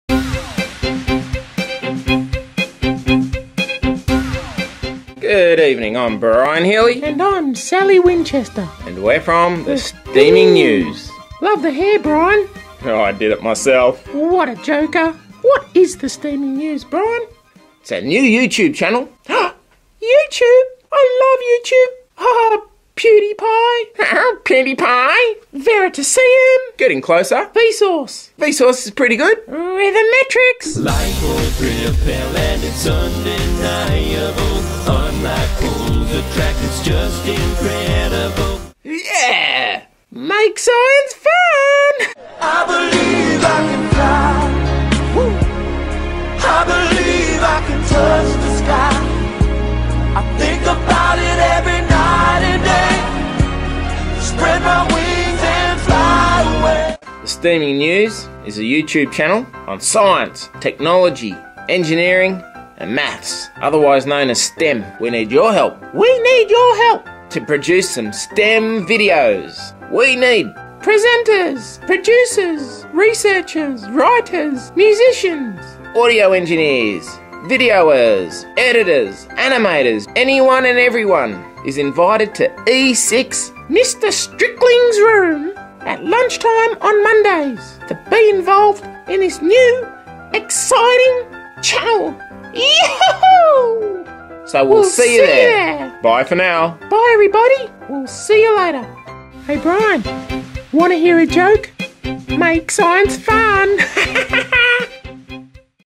First Assembly Presentation - The STEaMing News
This episode was aired at Green Point Christian College at Monday morning assembly 16th February 2015 to approximately 600 students from Years 7 to 12.
Its aim was to introduce the STEaMing News channel to the students and encourage them to be part of the production team.